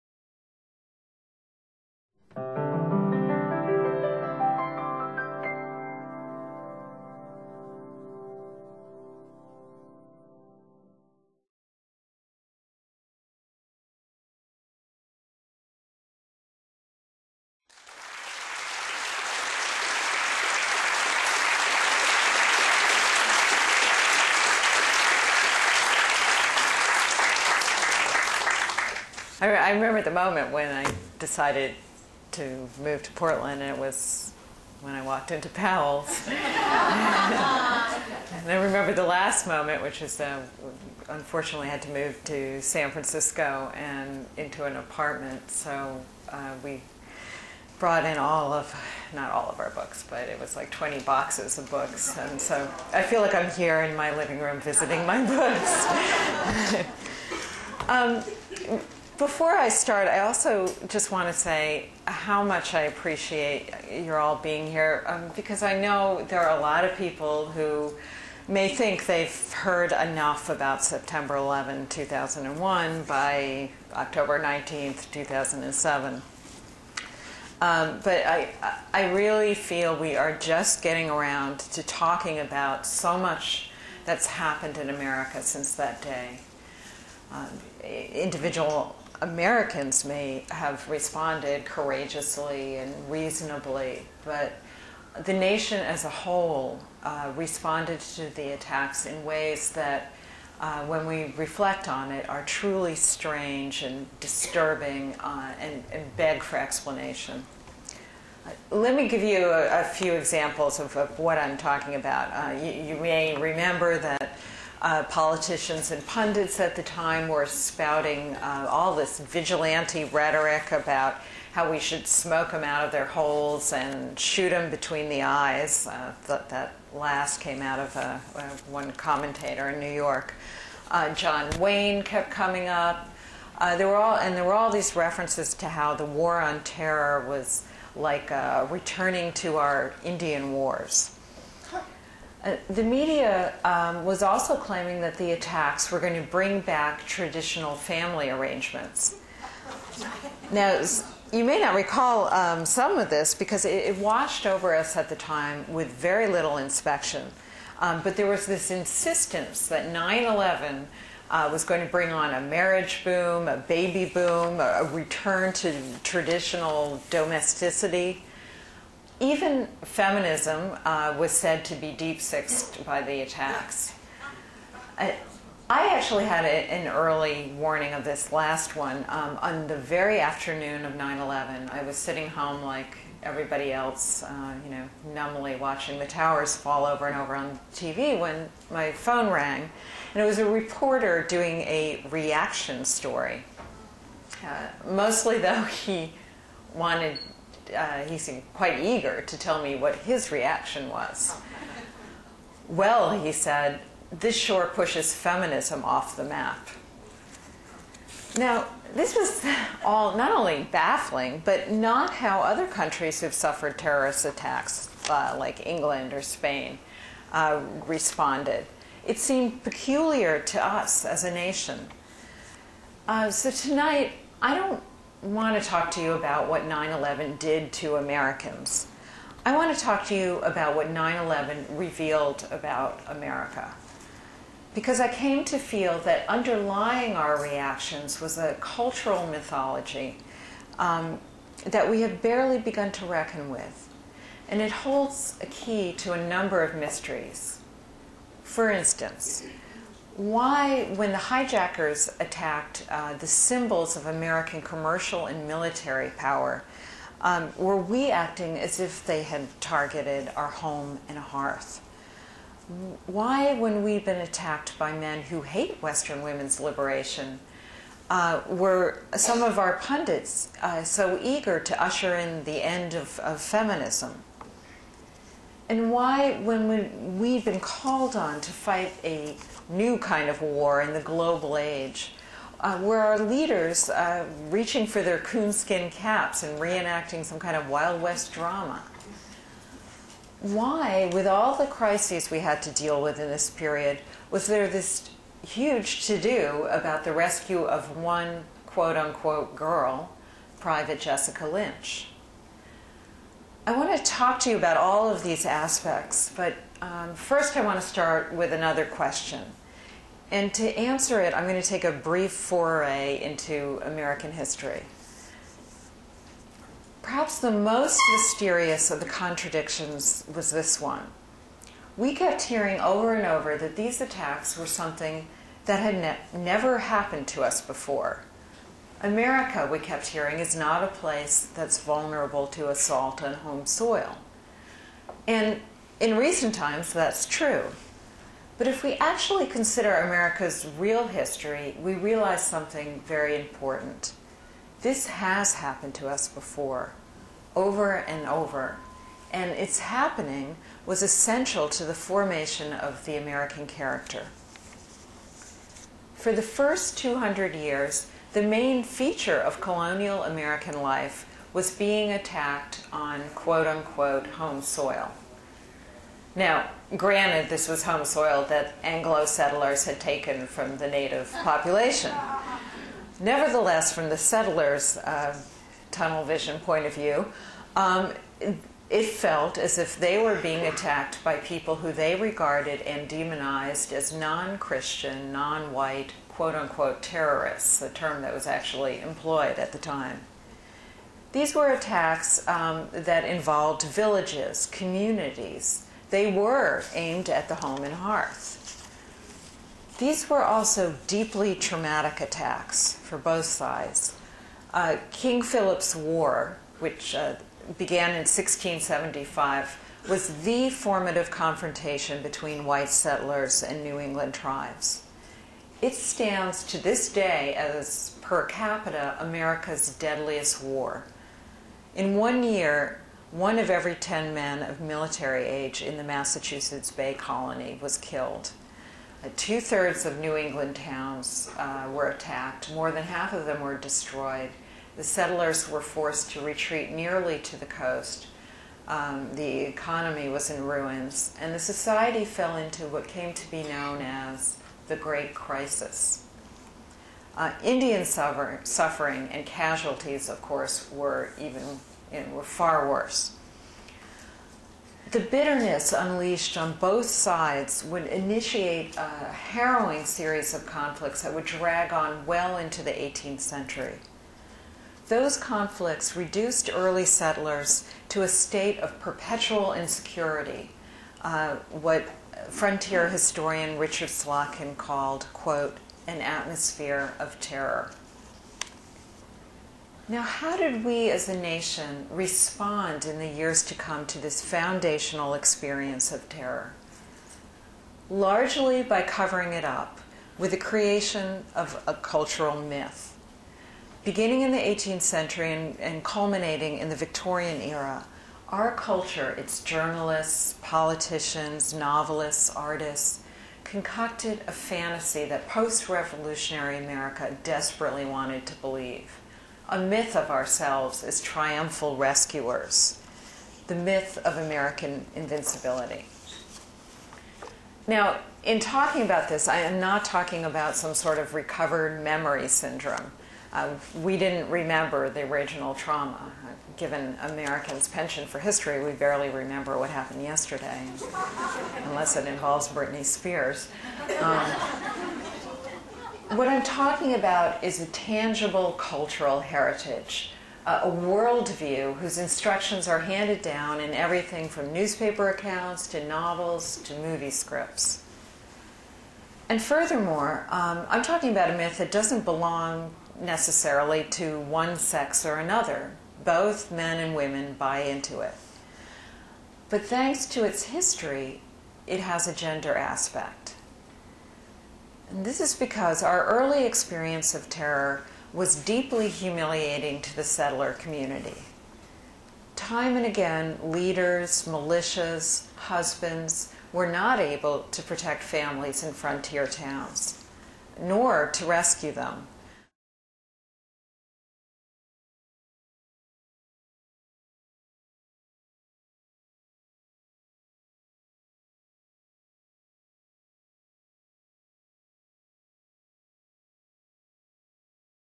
Susan Faludi - The Terror Dream
This video program is available in full and at the Google Video website (search on keywords "pdxjustice" and "Faludi").  The program's title is "The Terror Dream: Fear and Fantasy in Post-9/11 America."  Pulitzer Prize-winning journalist and author of the bestselling BACKLASH: THE UNDECLARED WAR AGAINST AMERICAN WOMEN, Susan Faludi, examines America's psychological response to the 9/11 attacks.